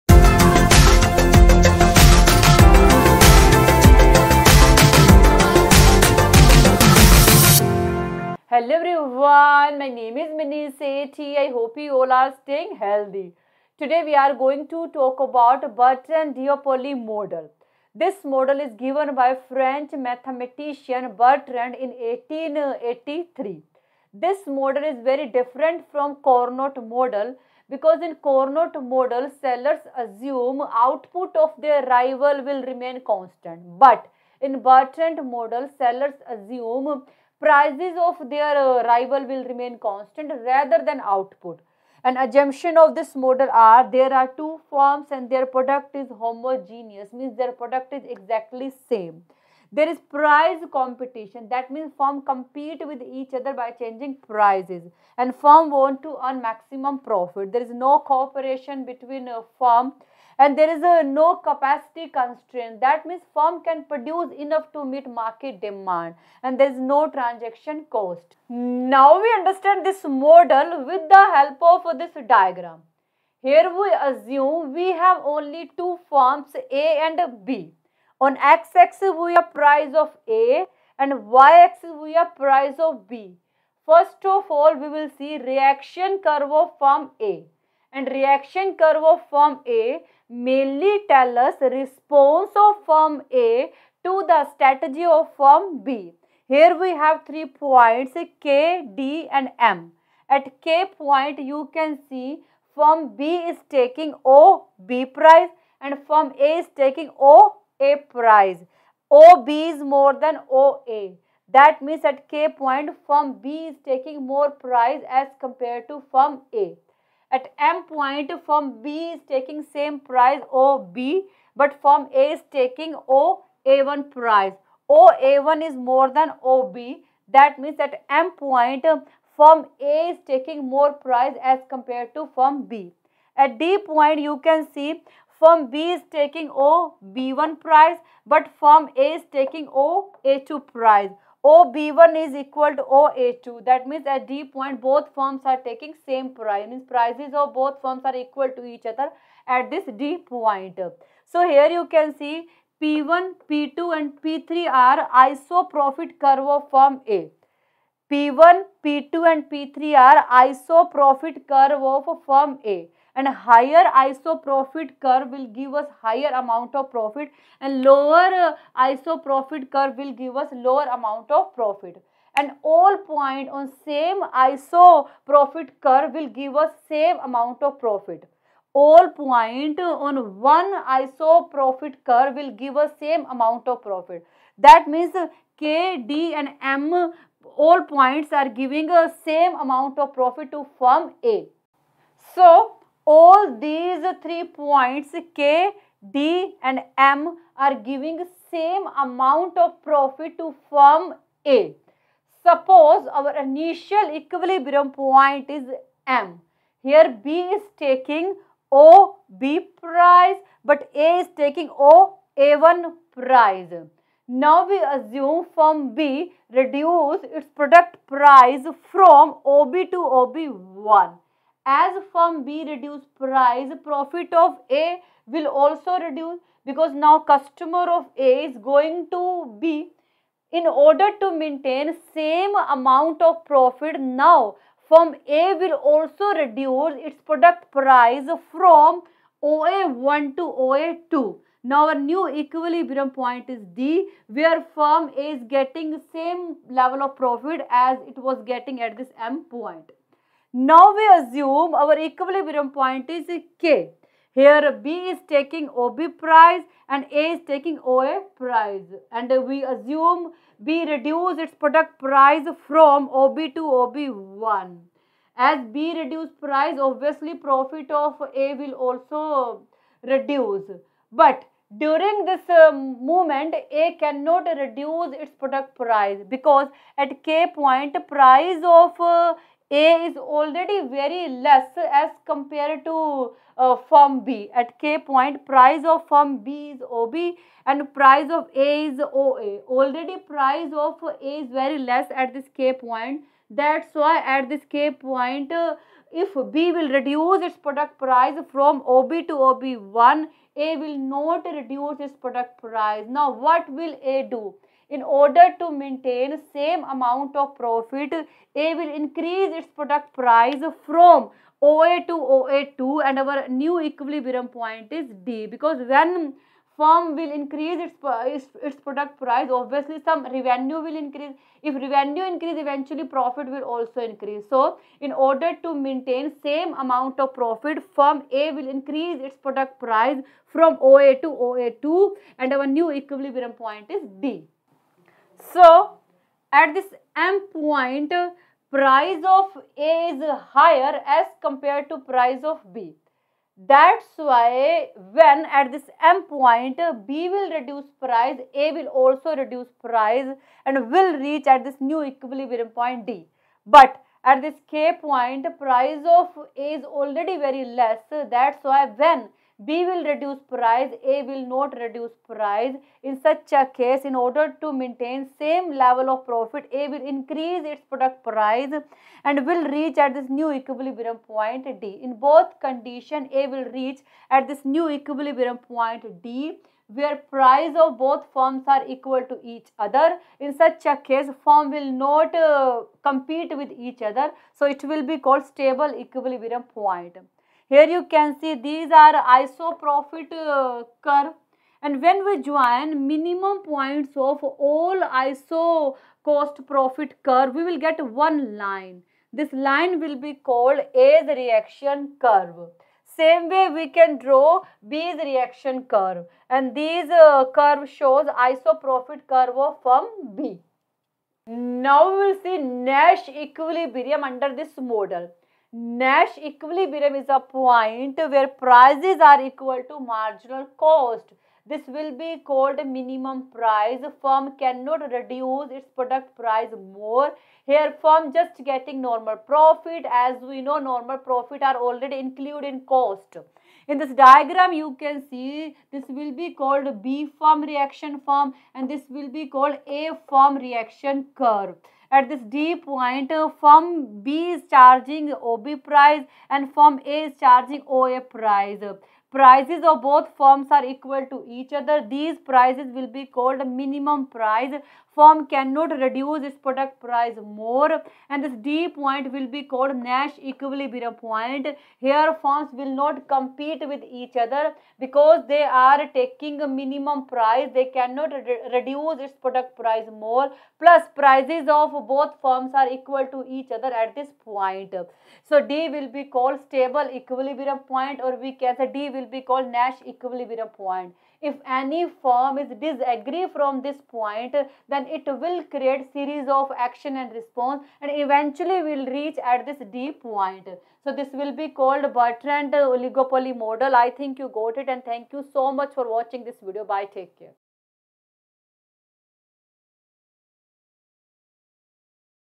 Bertrand's Duopoly Model
This video describes about Bertrand's Duopoly Model for UGC Net/JRF in economics